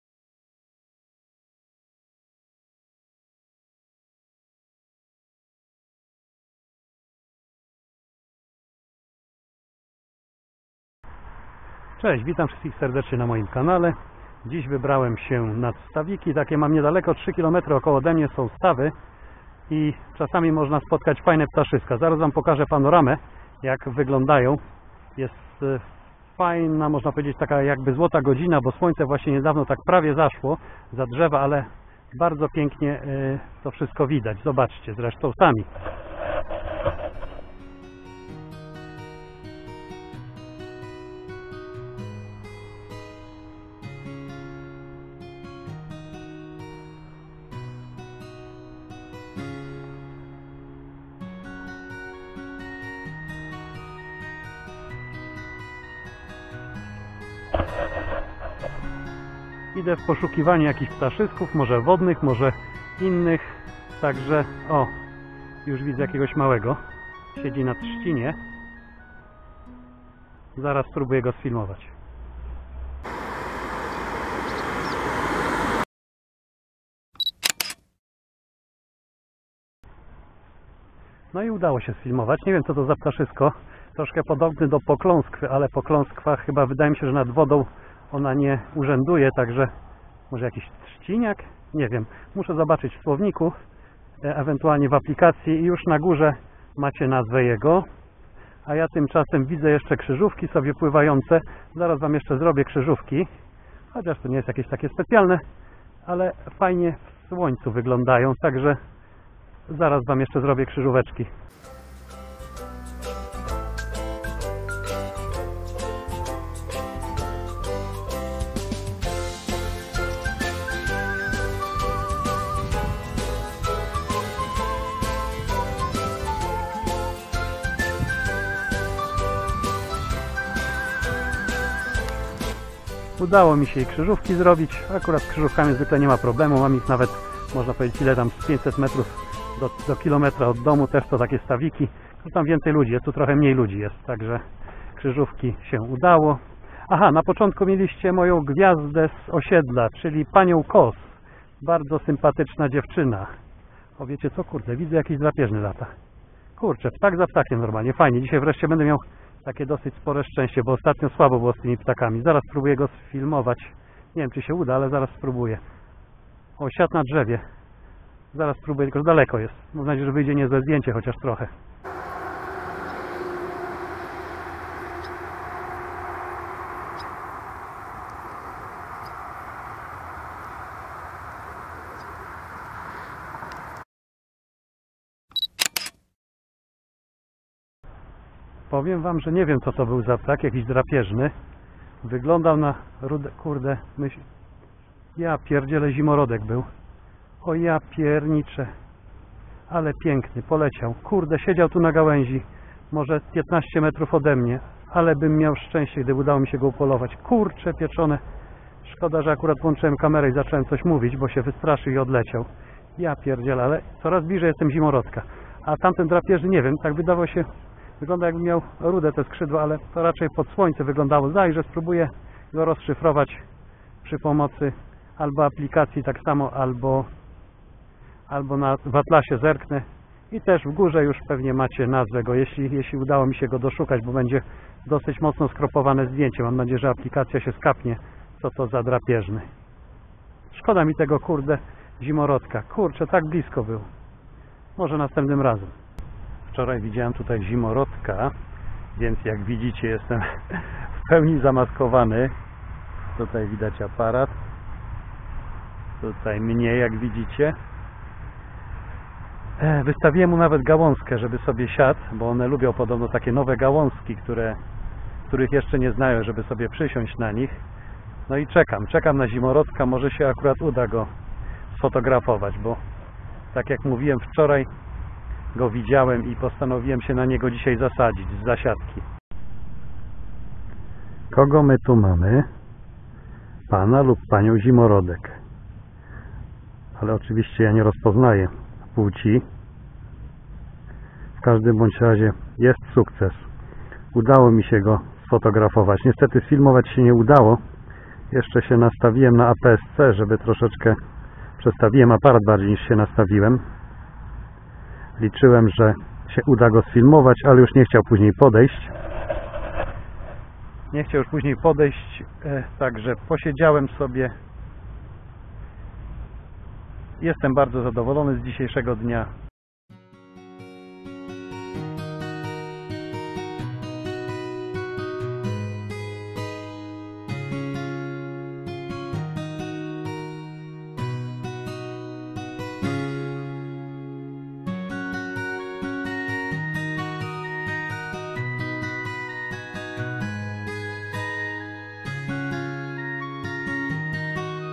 Zimorodek i nie Tylko Fotografia Ptaków
Udało się. Wreszcie. Zimorodek jest mój.
Zapraszam was do obejrzenia tego filmiku.
Jest na mim zimorodek i nie tylko.